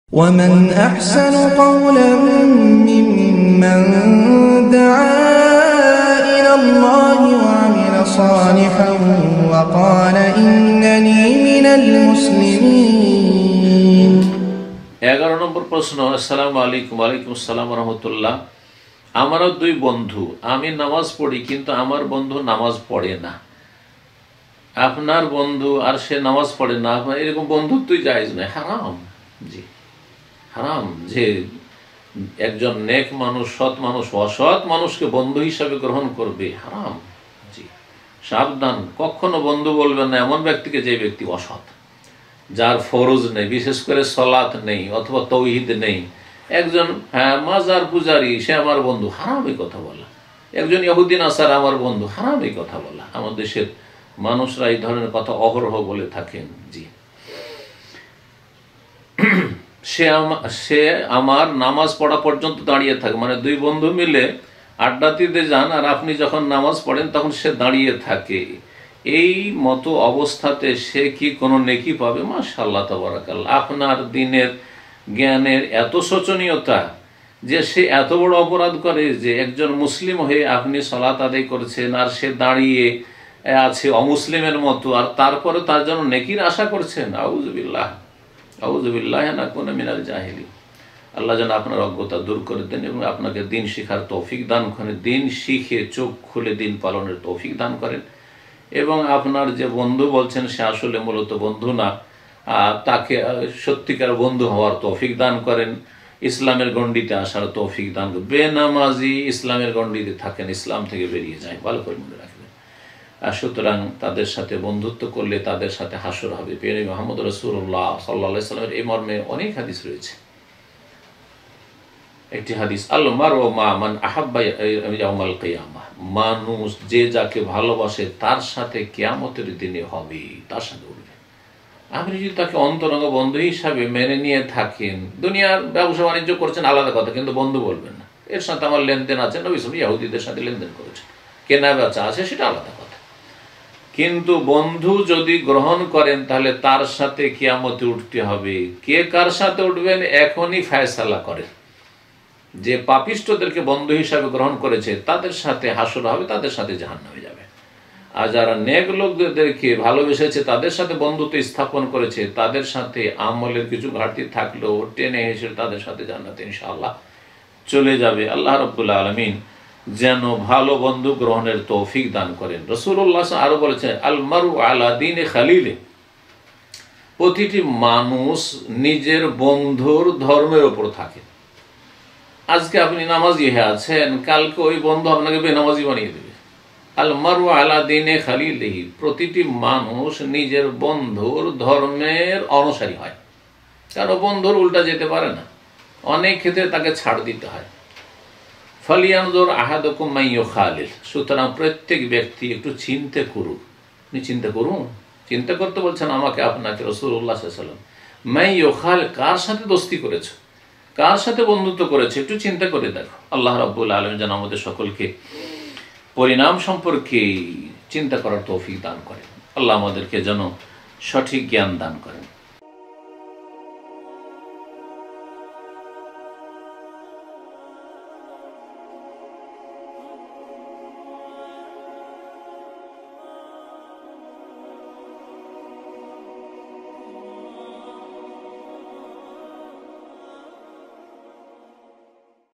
বন্ধু হিসেবে কাউকে গ্রহণ করার বিধান│শায়খ মতিউর রহমান মাদানী
▁▁▂▂▃▃▄▄► RB Media ◄▄▄▃▃▂▂▁▁
👨🏼‍💼Founder, Director & Producer:-  Jobrul Miah
📖 বিষয়ঃ-  বন্ধু হিসেবে কাউকে গ্রহণ করার বিধান
🎤 আলোচকঃ-  শায়খ মতিউর রহমান মাদানী
◆◆◆ Video Title ◆◆◆
◆◆◆ Related Hash Tag ◆◆◆
◆◆◆ Related Keyword ◆◆◆
 ◆◆◆ Download videos from this channel ◆◆◆